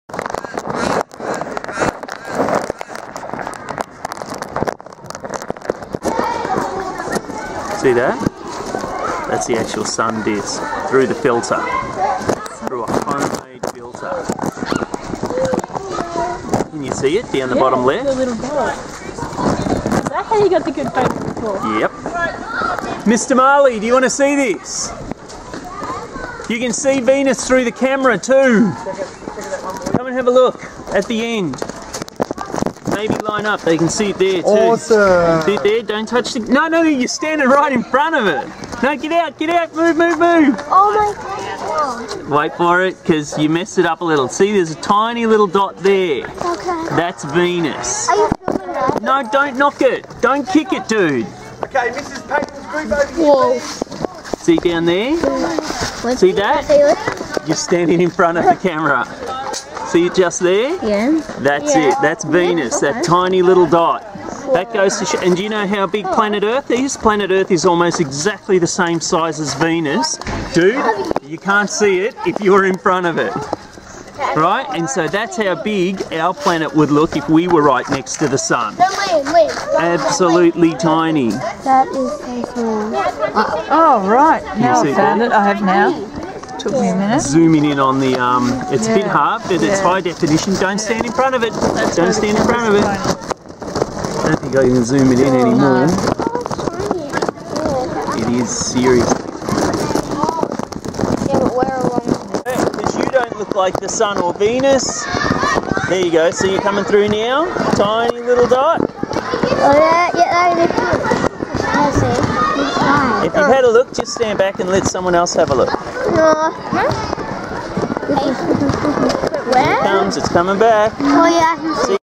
Transit of Venus
We filmed the transit of Venus at school! It was so cool! We filmed the event using Panasonic HDC-SD40.